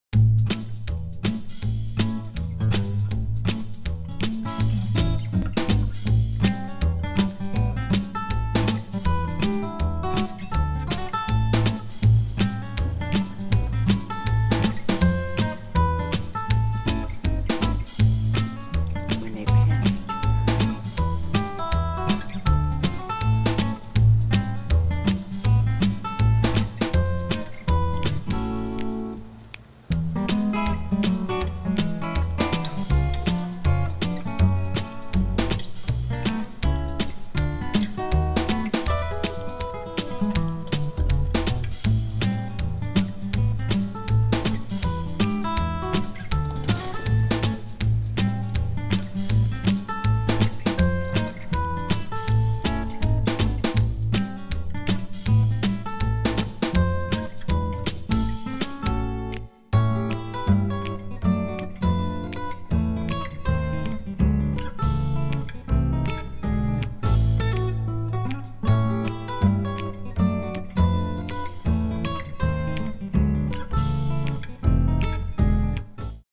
Cats' balance beam
Young indoor cats insisting on getting as high as they can and challenging their sense of balance.  Heart-stopping to watch for those who love them.